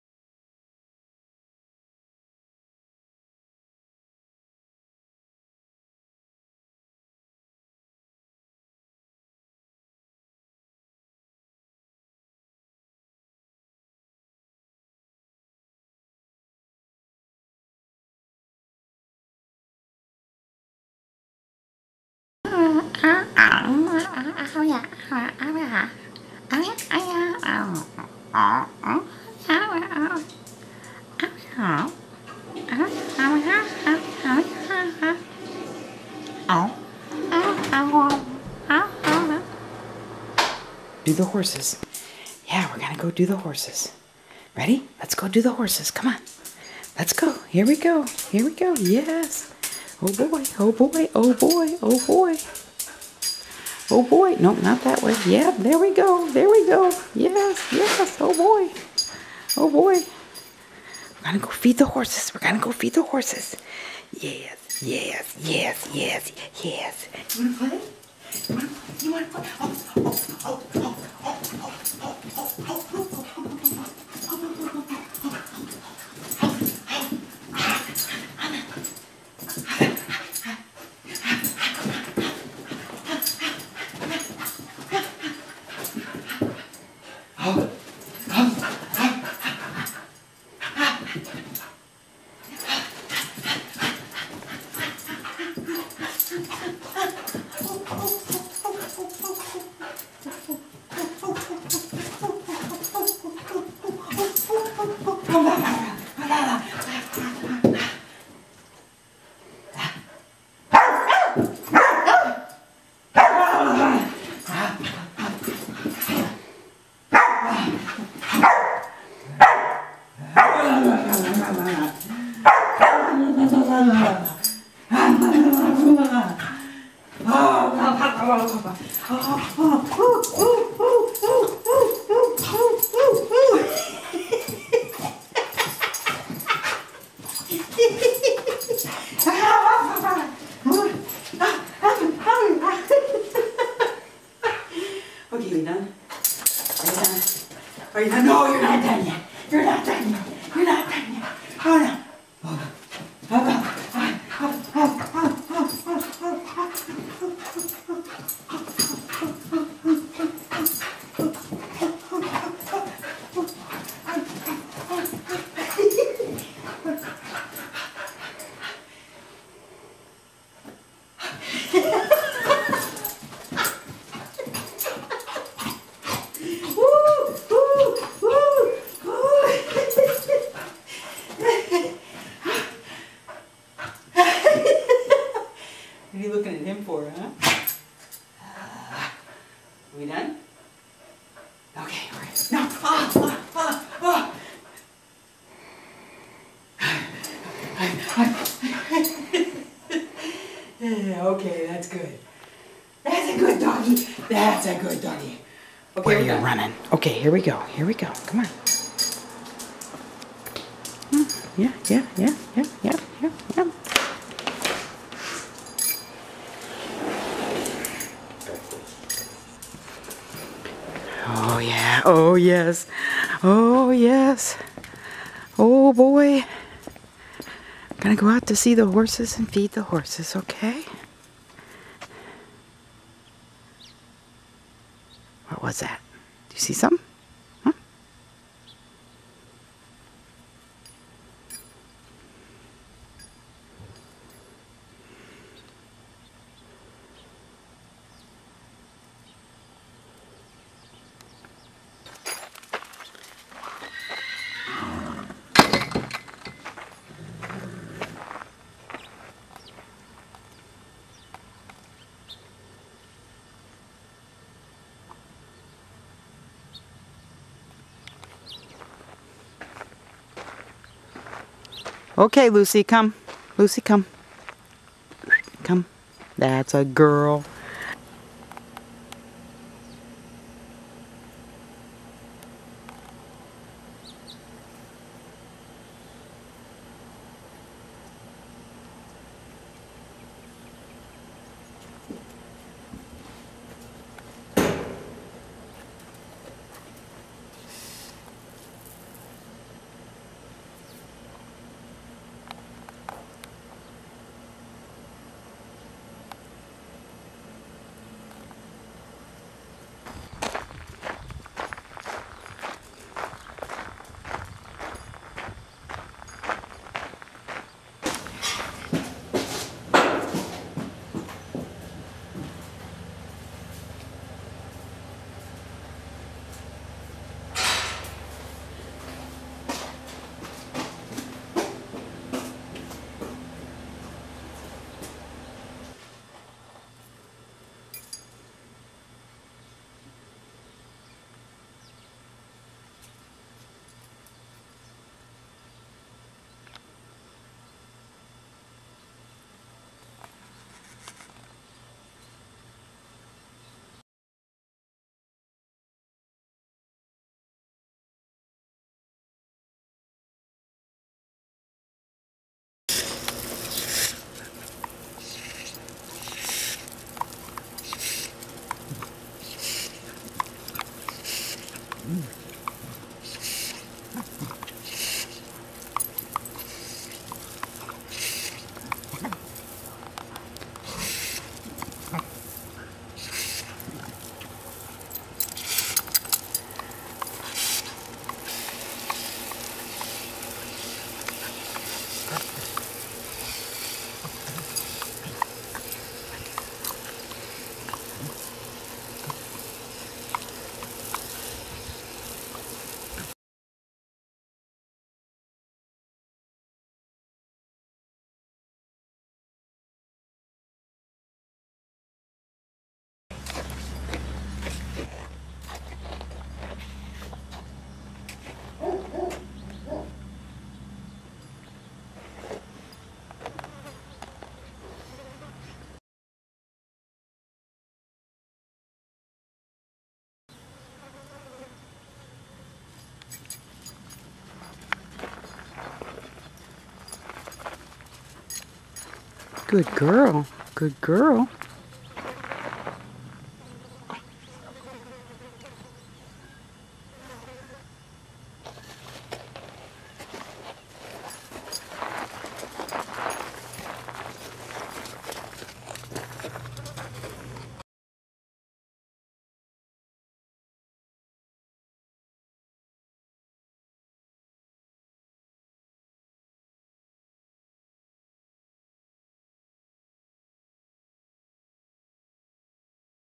HSNT Lucy Tripod's Debut
Thanks to the Humane Society of North Texas for Lucy Tripod, an Australian Cattle dog mix, that we adopted on August 14, 2010 at the Mutt Makeover in Ft. Worth!  Lucy is 5 years old and shows you that a three-legged dog can do whatever he or she puts their mind to.